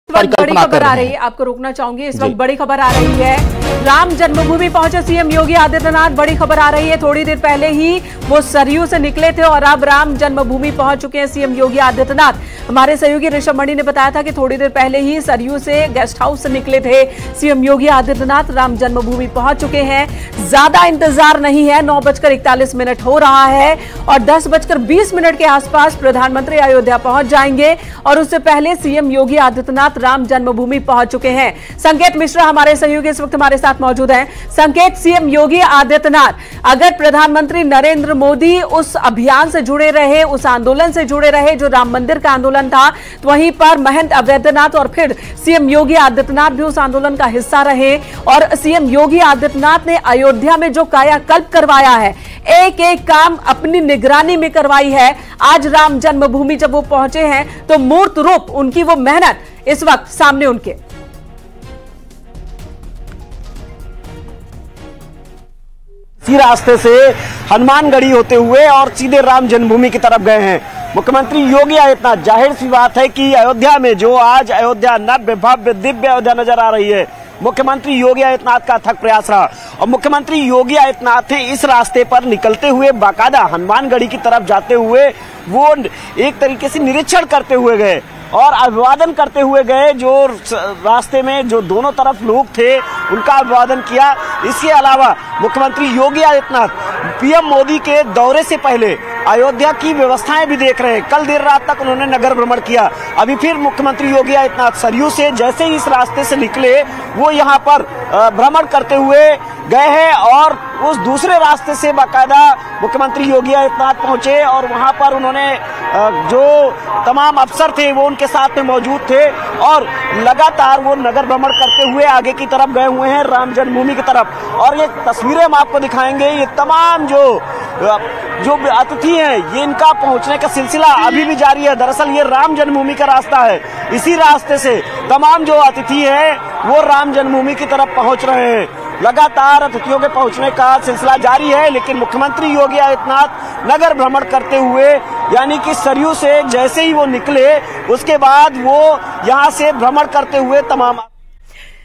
Breaking News | कड़ी सुरक्षा में राम मंदिर पहुंचे CM Yogi | Ayodhya Ram Mandir News | Pran Pratishtha
Breaking News | कड़ी सुरक्षा में राम मंदिर पहुंचे CM योगी | Ayodhya Ram Mandir News | Pran Pratishtha